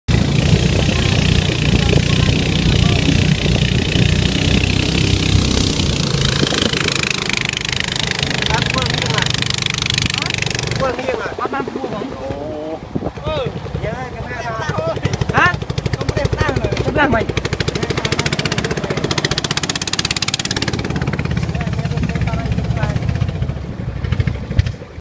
Giant Crab on the boat to Tonle Sap Lake, Cambodia !!!អាយ៉ាស់ ! ក្តាមយក្សតើ!
Crab is the kind of living  thing on the earth . Crabs has been  not just living  only in river but also in the Ocean . Crabs eat small fishes , some kind of grasses in the bottom of the river. In Cambodia we have meet many kind of crabs still alive now , especially in the field rice. if you visit Cambodia don't forget to see the sun set and catch the crab at the field rice or go to visit hug crab in Tonle Sap lake .  don't forget to subscribe this youtube channel to get more info about Cambodia Amazing Fact !